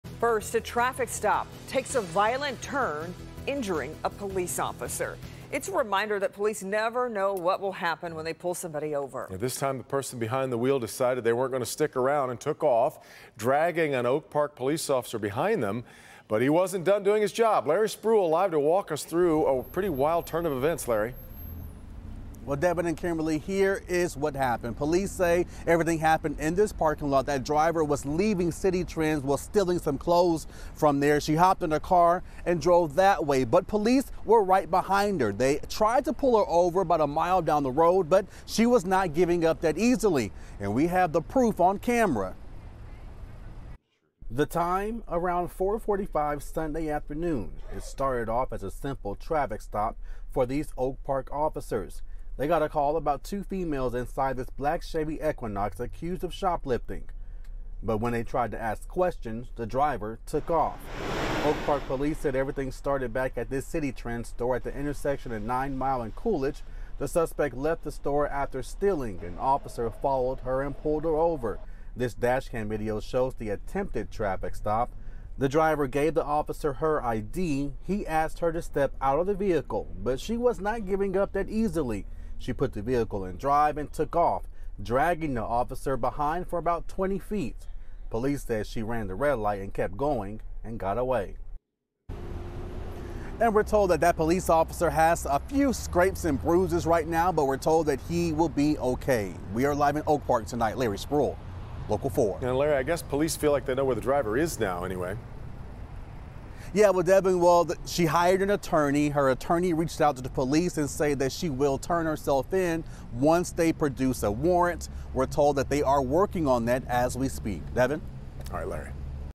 Video shows Oak Park police officer being dragged by driver fleeing traffic stop
An Oak Park police officer was dragged by a driver trying to flee a traffic stop.